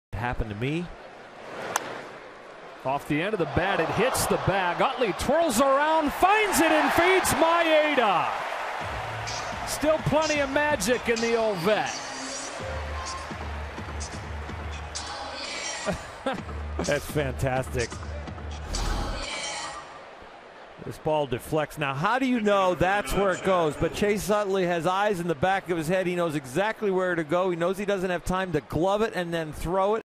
PIT@LAD: Utley makes a nice play on a tough grounder
Chase Utley fields the ground ball as it bounces off the first-base bag and flips the ball to Kenta Maeda for the quick out